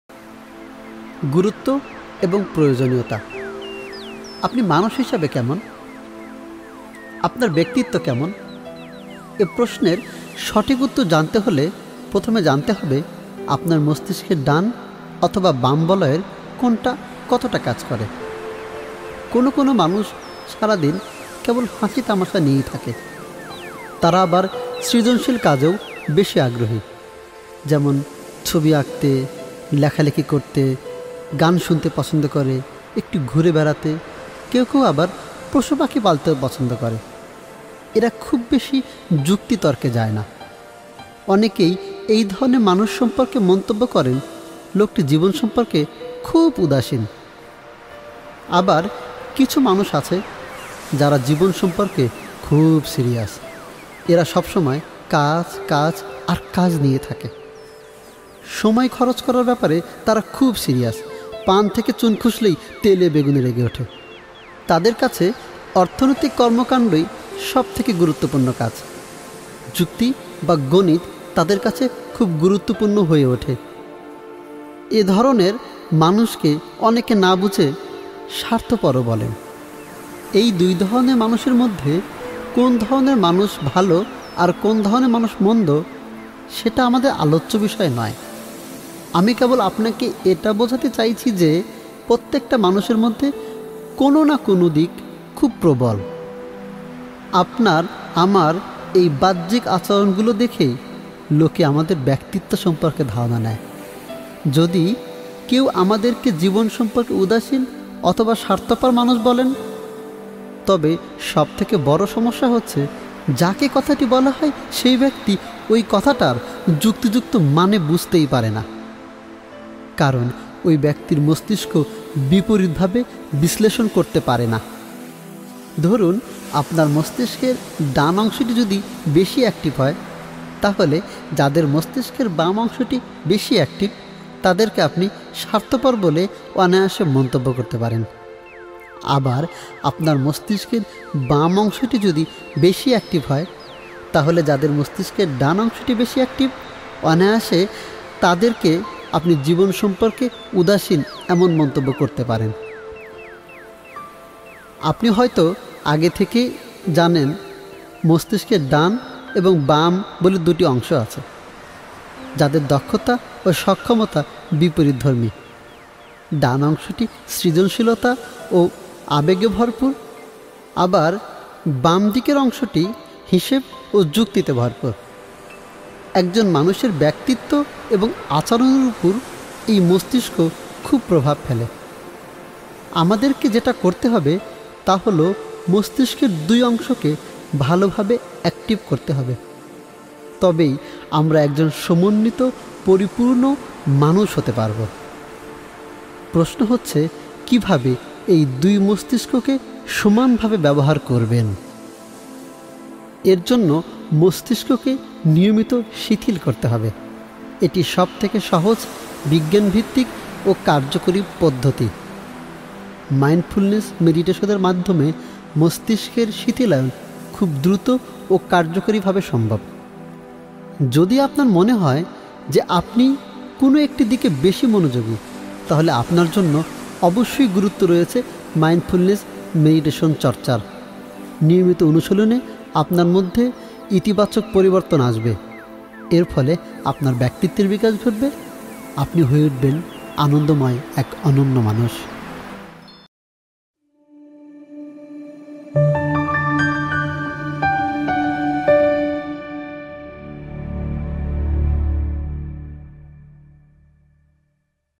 Mindfulness : গুরুত্ব এবং প্রয়োজনীয়তা? || কেন সারা পৃথিবীতে মাইন্ডফুলনেস চর্চা করা হয়?
কেন মাইন্ডফুলনেস চর্চা করা দরকার? কেন সারা পৃথিবীতে মাইন্ডফুলনেস এটি চর্চা করা হয়?
Mindfulness : গুরুত্ব এবং প্রয়োজনীয়তা?

আপনি মানুষ হিসেবে কেমন? আপনার ব্যক্তিত্য কেমন?
এ প্রশ্নের সঠিক উত্তর জানতে হলে জানতে হবে আপনার মস্তিস্কের ডান ও বাম বলায়ের কোন অংশ কতটা কাজ করে।